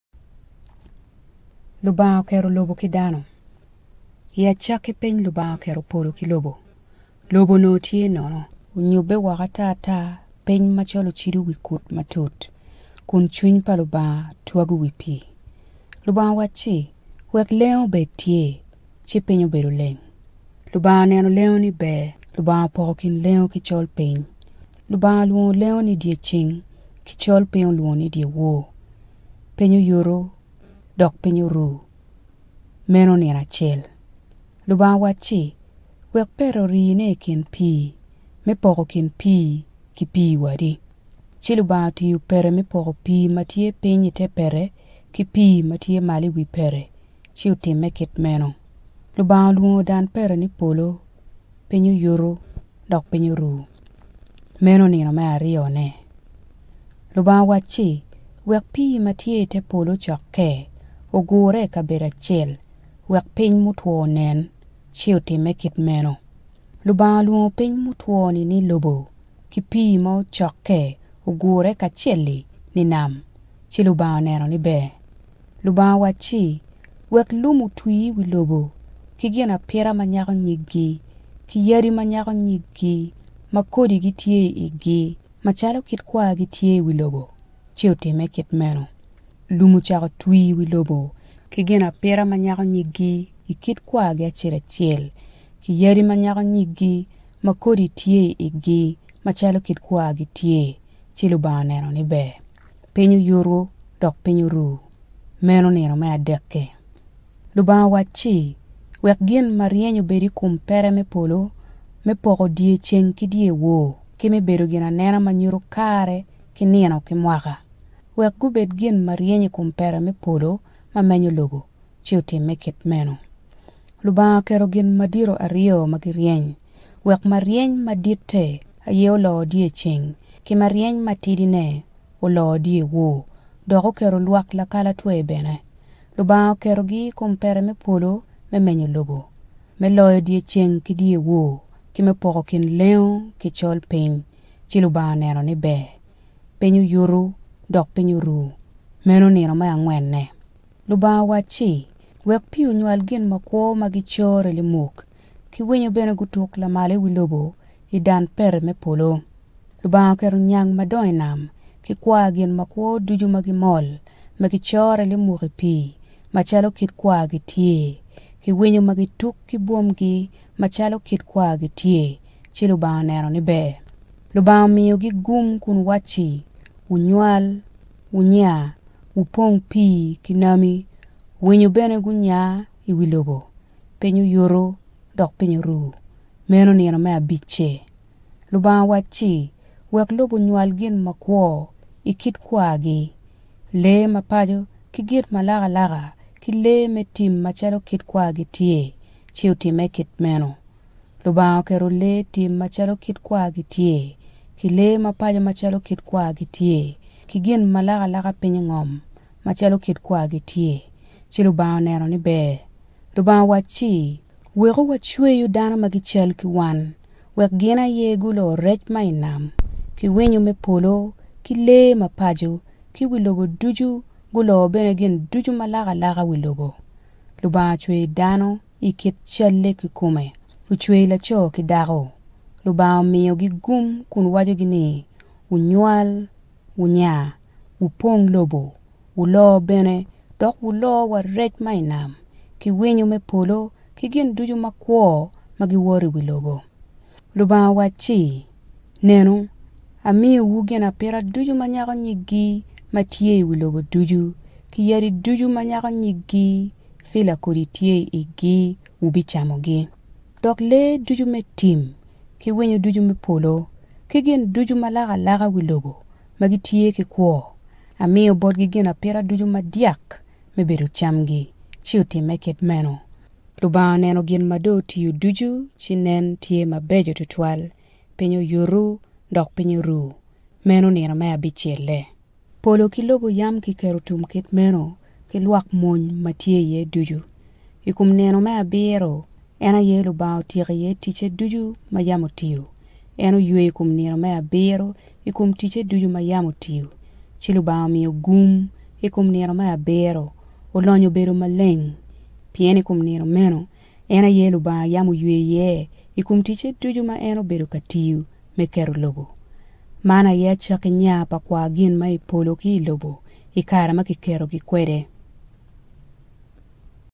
Acaki - Lubanga oketo lobo ki dano
Genesis - the first seven days of creation.